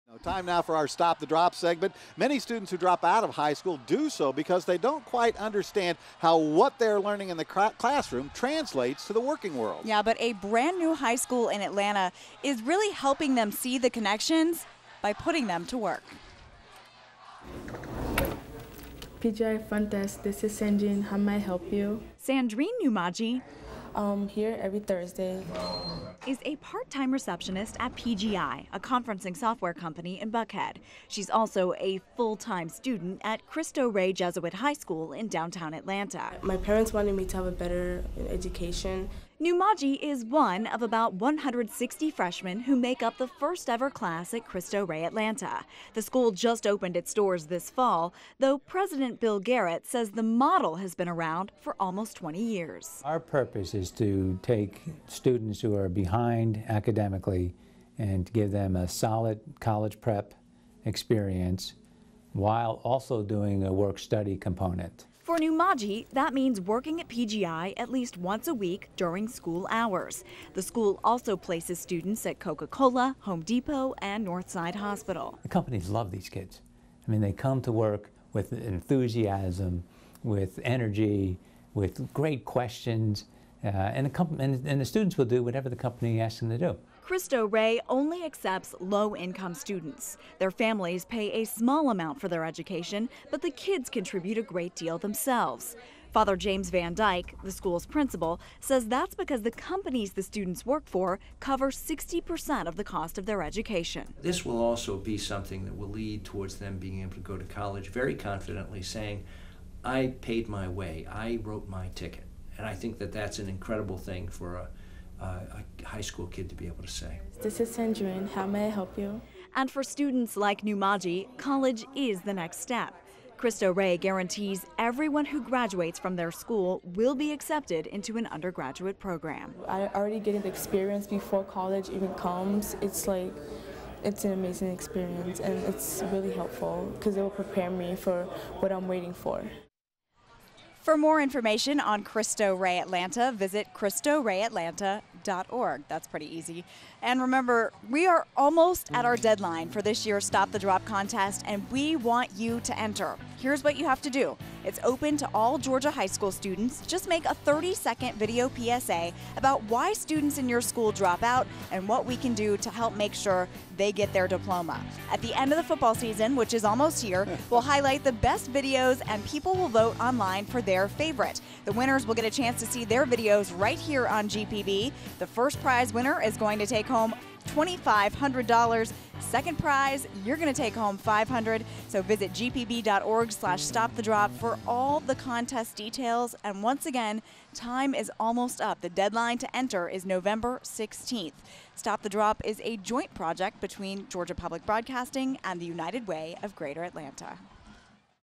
Stop The Drop: Cristo Rey Atlanta
GPB's Claire Simms features a student at Cristo Rey Atlanta, a brand new school that opened for the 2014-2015 school year.  Cristo Rey combines a rigorous math and English curriculum with real-world experience.

Stop The Drop is a joint initiative between Georgia Public Broadcasting and The United Way of Greater Atlanta.